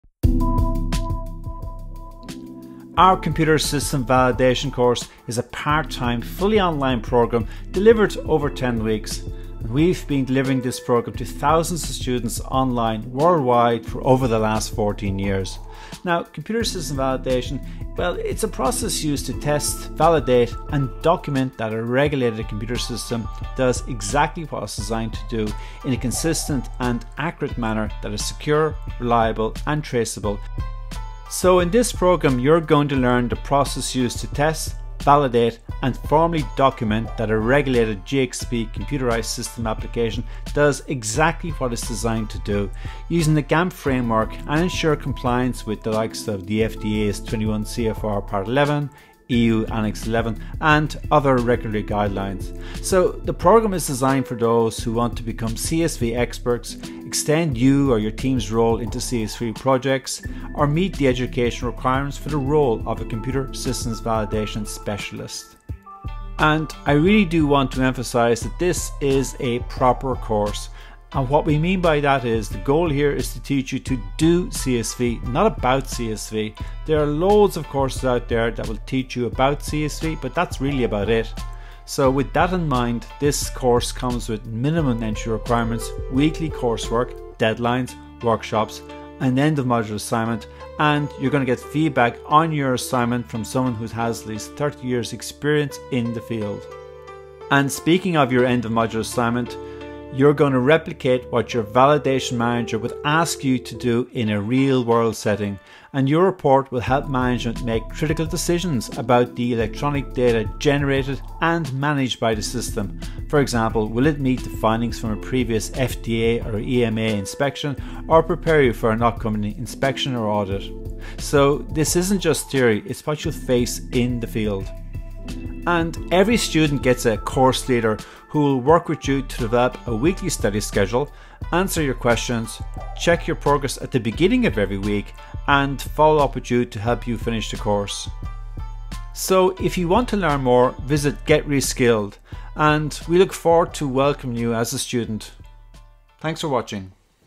Computer System Validation (CSV) Training Course - GetReskilled
Extend Your Role to CSV Projects. Earn a GxP Computer System Validation Certificate. Become a CSV Professional

Has the Computer System Validation Engineer left and you’ve been handed their responsibilities? Are you dreading your next inspection? CSV can be frustrating but this program will show you how to manage electronic data in a regulated manufacturing/laboratory environment using the GAMP framework and ensure compliance with FDA’s CFR Part 11, EU Annex 11 or other regulatory guidelines.

Learn the process used to test, validate and formally document that a regulated GxP computerized system application does exactly what it is designed to do

Extend your or your team’s role into CSV projects. Charge higher hourly rates

Go rapidly from beginner to advanced level.